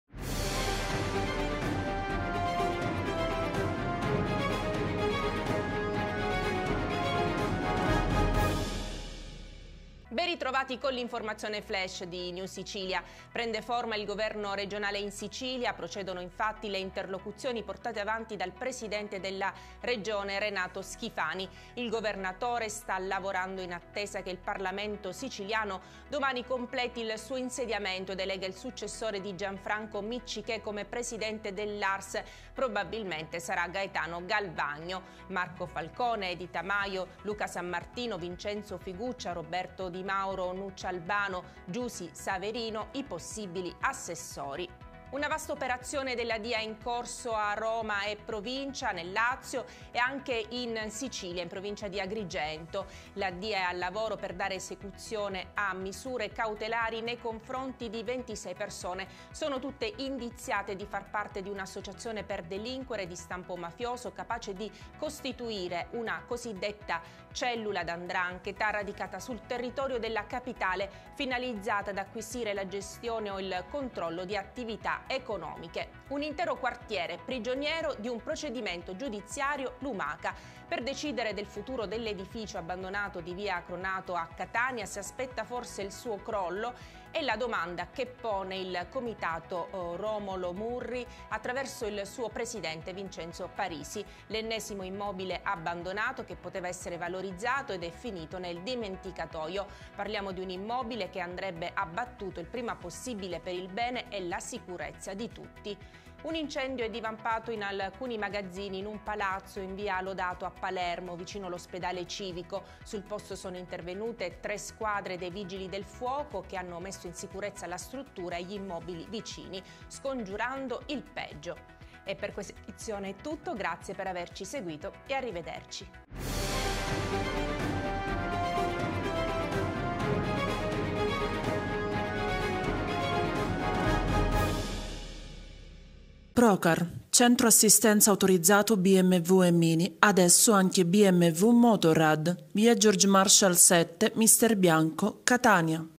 Tg Flash edizione del 9 novembre delle ore 10,00
Edizione del 9 novembre 🗓 delle ore 10,00 🕒

• Prende forma il nuovo governo in Sicilia: i nomi dei possibili assessori ➡️

• Vasta operazione della Dia a Roma, coinvolta anche la Sicilia: eseguite 26 misure cautelari➡️

• Palazzo via Cronato a Catania pericolo per l’intero quartiere, l’allarme lanciato dal comitato Romolo Murri➡️

• Incendio in alcuni magazzini di un palazzo vicino l’ospedale Civico: 3 squadre di vigili del fuoco sul posto ➡️